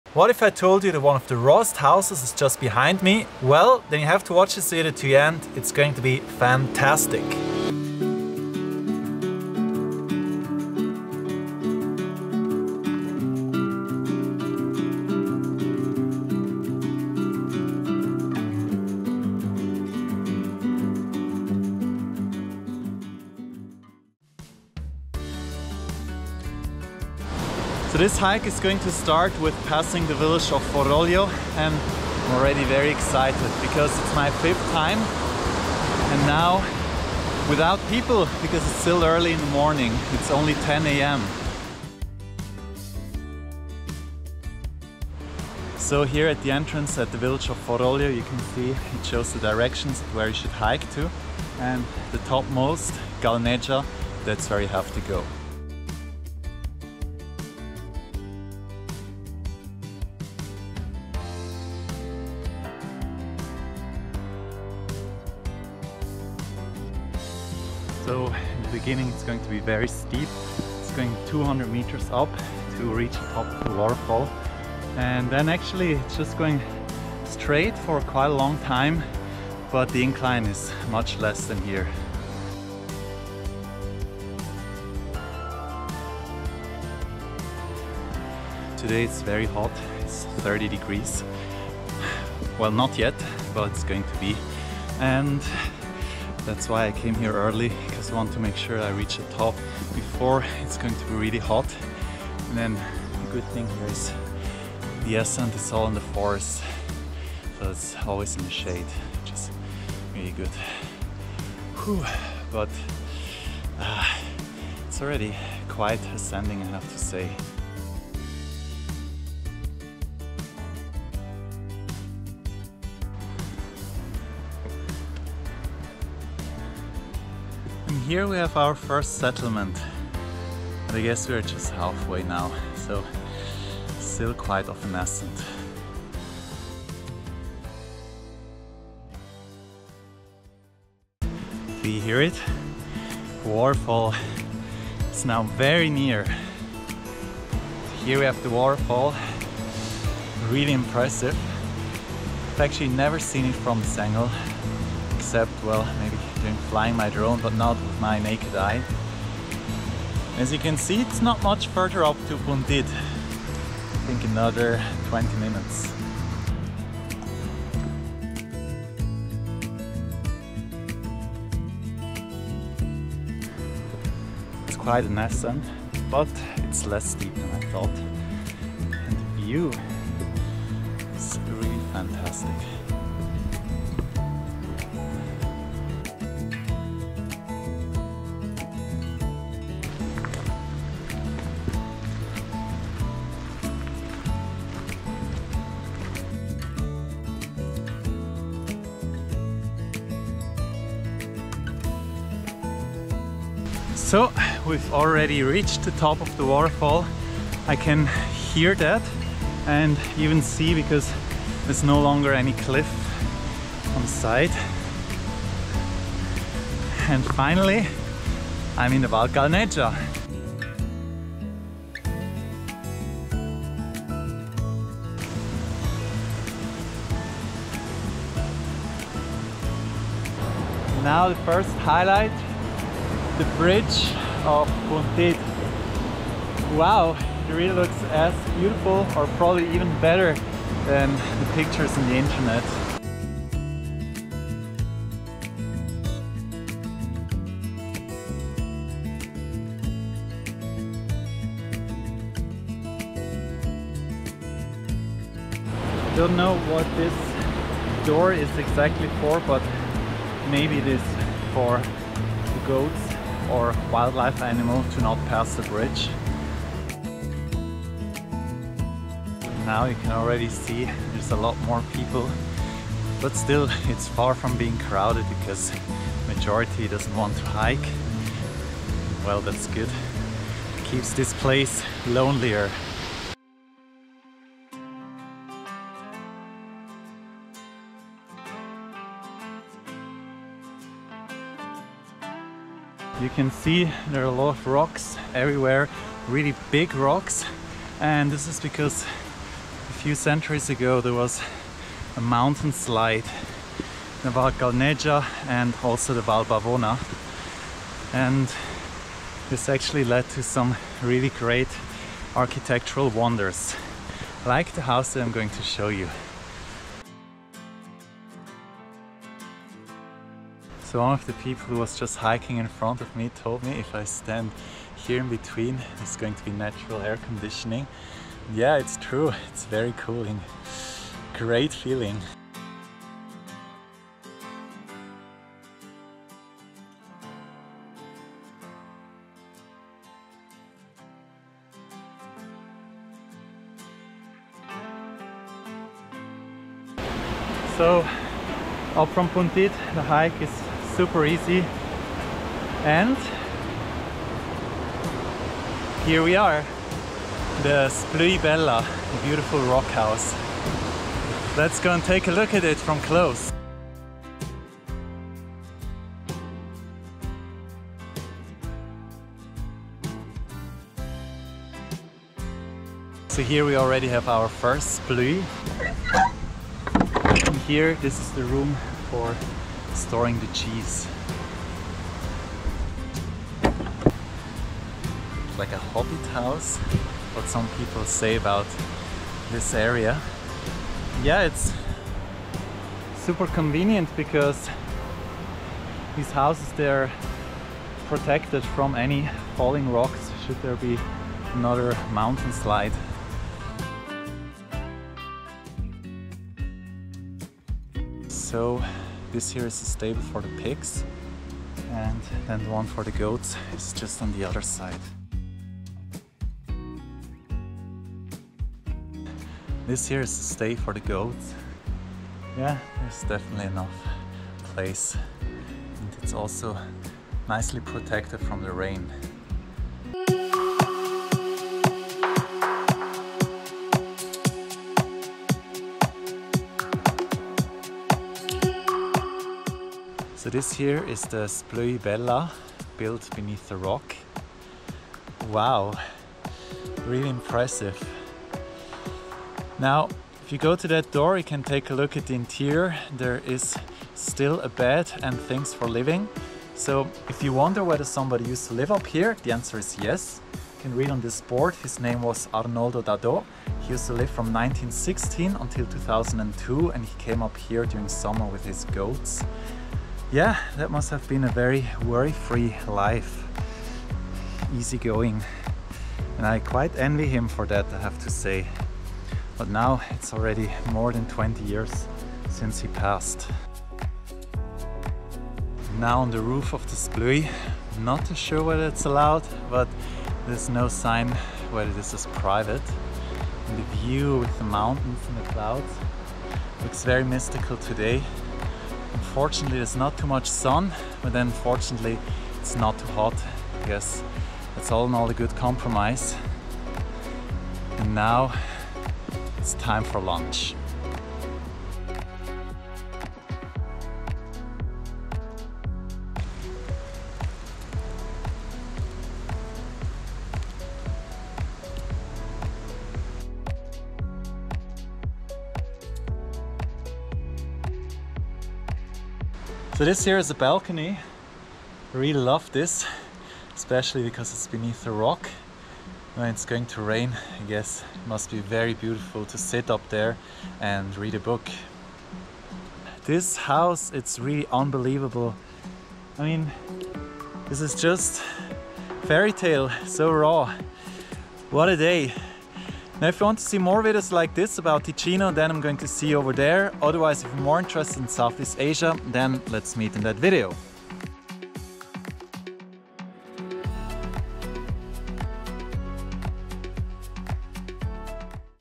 The CRAZIEST & Most UNIQUE Swiss House – HOW to FIND it!
Switzerland charms with its Alps and many traditional villages and houses. There is this secret valley in Ticino that is a true paradise for hikers who love to explore the most remote areas of Switzerland. Sometimes, the best secrets of Switzerland lay in the less visited corners, such as at this lonely place in the Swiss Alps. It takes roughly 1 and a half hours to hike up to this crazy and unique house. The first part of the hike is steep, the ascent is roughly 200 meters to reach the village just at the top of the waterfall. You then will arrive at a small settlement by the creek that charms with a wonderful bridge that makes one of the best photo motifs of Ticino. The bridge at the folk village of Puntid. The hike then goes along the creek, it’s certainly a lot easier. The trail will lead you through numerous rocks that fell during a mountain slide that happened centuries ago. This mountain slide is the reason, why many of the locals built their houses beneath the rocks. These unique folk houses are like Hobbit houses, they give a safe shelter and certainly are more protected against any mountain slides than the previous ones. These unique hobbit houses are called Splüi by the locals. The roofs of these houses are made by the rocks themselves. The Splüia Bela is the most unique and craziest of these typical houses of Ticino. Although it may take quite a while to reach, the hike is very much worth it. The tranquility at the house, the loneliness and the connection to the nature and the Swiss Alps is unbelievably beautiful.

Equipment: Sony α6500, GoPro Hero 10 Black, DJI Mini 3, DJI Mavic 2 Pro